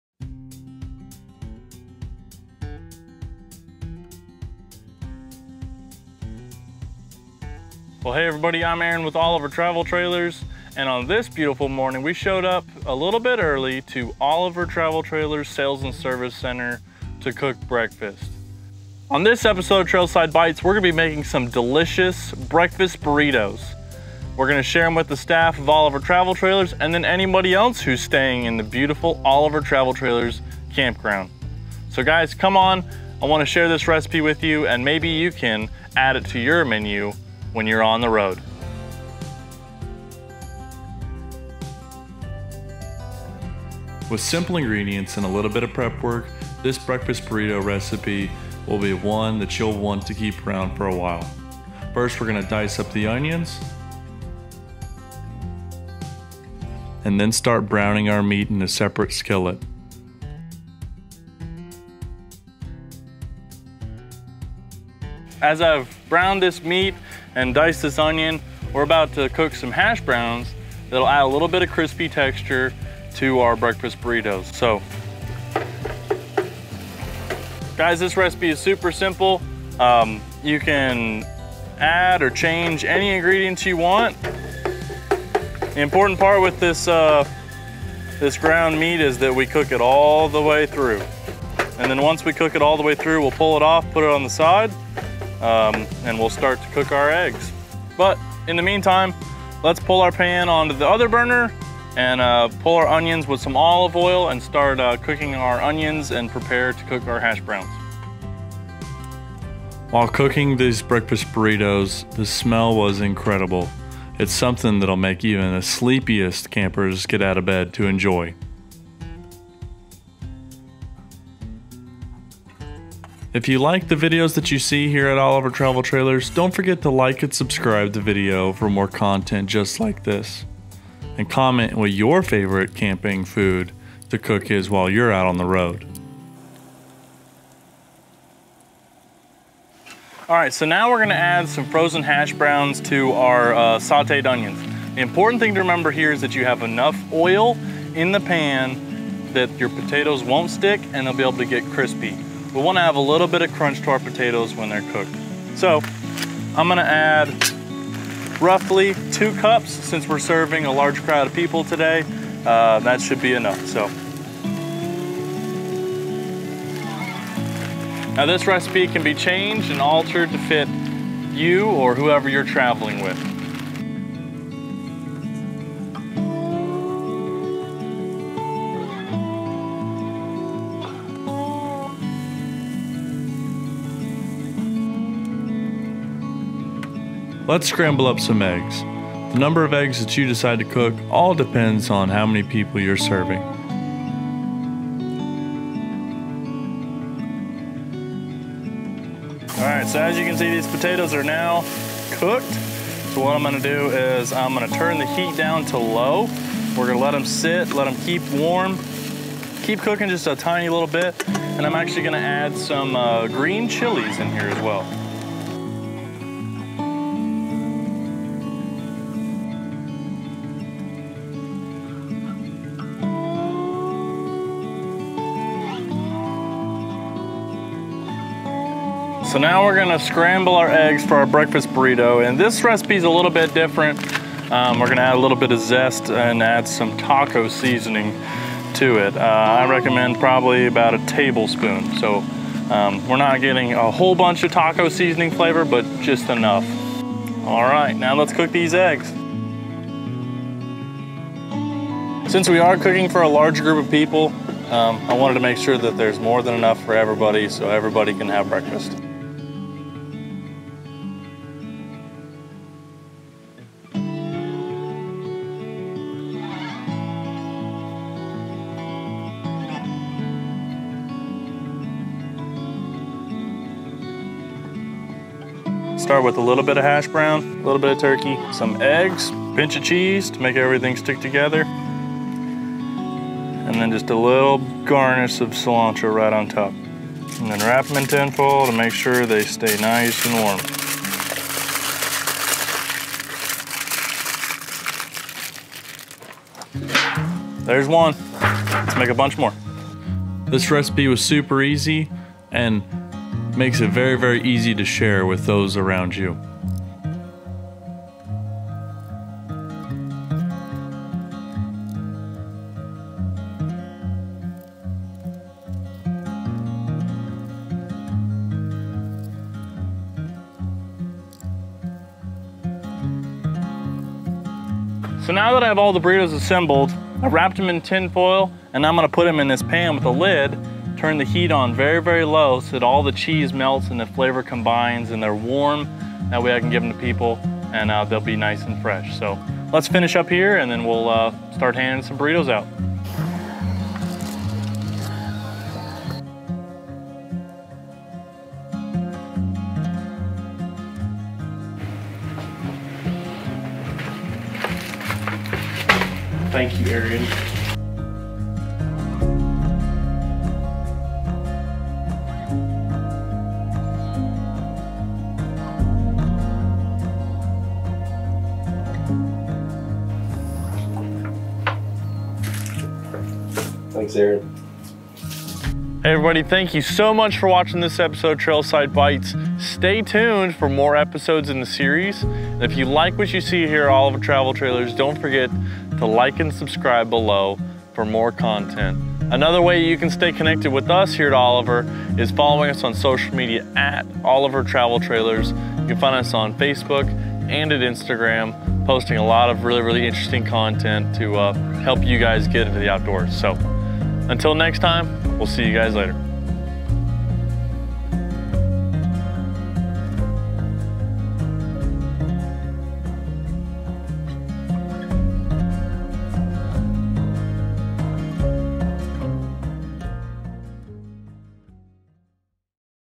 Breakfast Burritos | Trailside Bites S2 E1 | Oliver Travel Trailers
Get ready to rise and shine with TrailSide Bites Season 2, Episode 1 - Breakfast Burritos!

Join us on an incredible adventure that combines the magic of Oliver Travel Trailers, the joy of trailer living, and mouthwatering camping food. In this exciting Season 2 opener, we're bringing you breakfast burritos as epic as your outdoor exploration. 🌄🌯

Experience the sizzle of the skillet and the first satisfying bite as we infuse adventure into every fold and fill. We'll show you how to turn simple ingredients into a burst of morning delight, perfect for any adventure-loving soul.

Take advantage of all the fun and flavors in this must-watch episode of Trailside Bites.